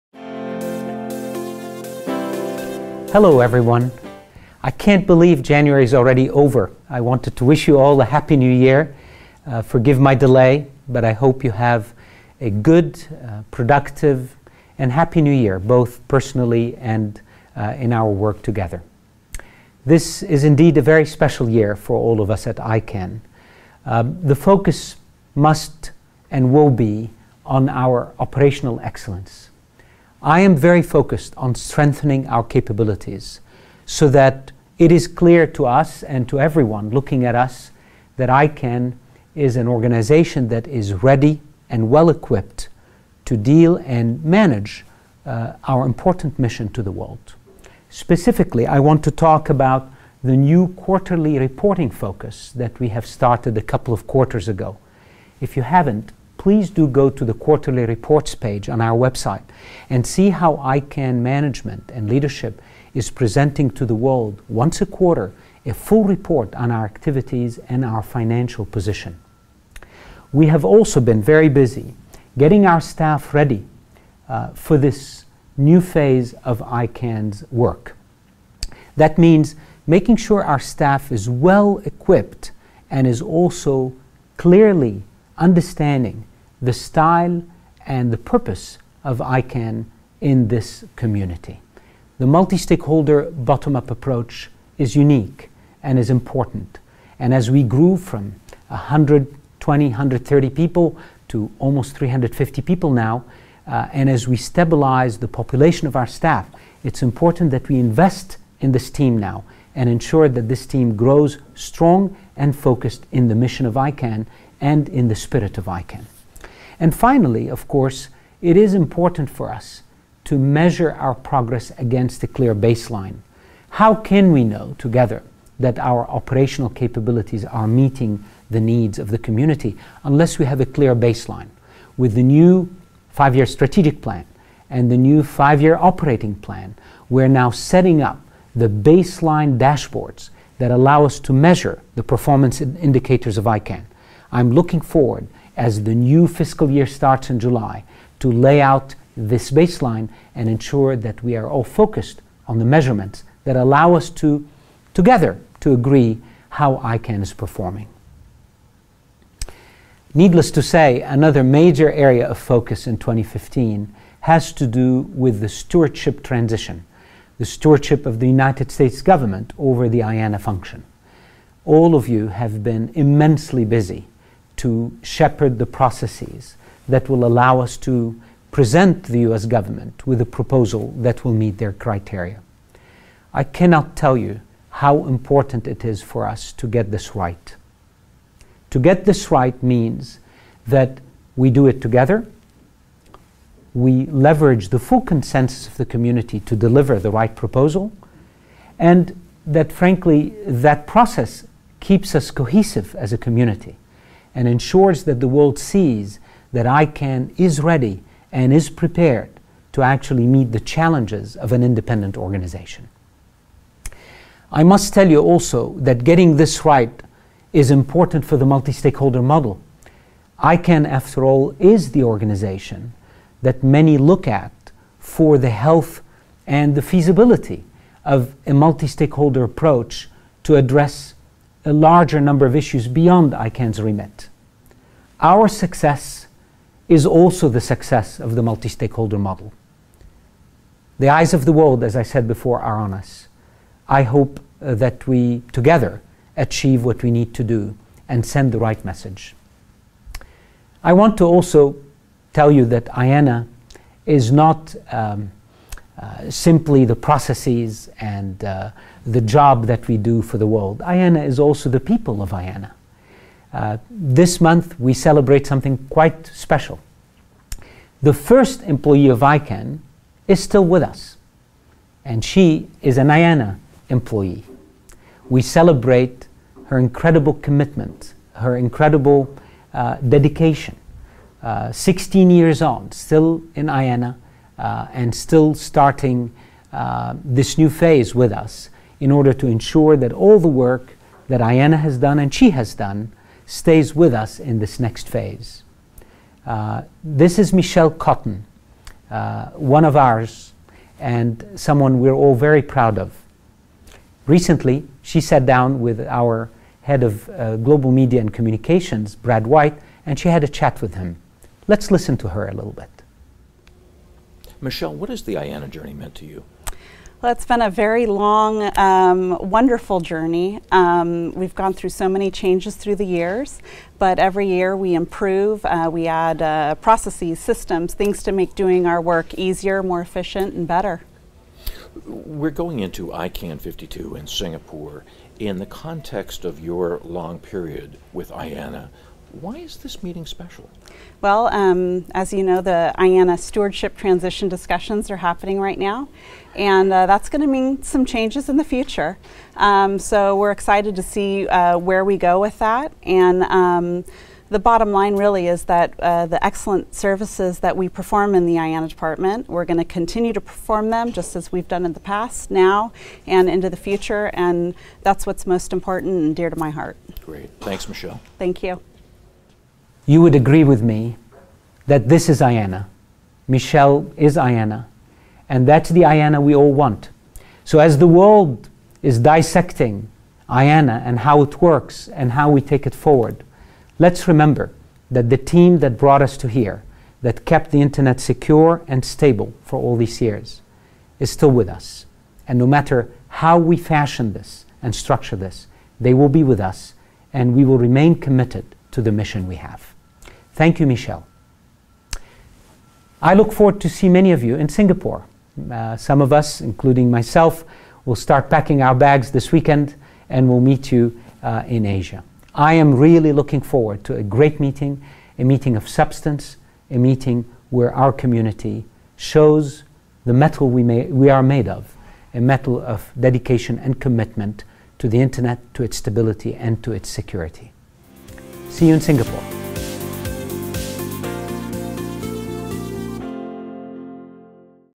Countdown to ICANN 52: ICANN and the Year Ahead
Fadi Chehadé, ICANN President and CEO, sends his Happy New Year wishes to the community for 2015 and looks forward to ICANN 52 and beyond in this video message.

This year’s focus will be strengthening ICANN’s operational excellence and capabilities to deliver our mission to the world.  This will be accomplished through quarterly reporting, further equipping and investing in our staff, and measuring organizational progress against a clear baseline.

The IANA stewardship transition will be at the forefront this year as a test of the multistakeholder model by which ICANN operates and as a test of our community.  It is important to remember that IANA encompasses not only the technical functions of that department but also the people who support its critical operations.  In this spirit, we recognize the 15th anniversary of team member Michelle Cotton and her longstanding, dedicated service with ICANN.

We hope to see you in Singapore!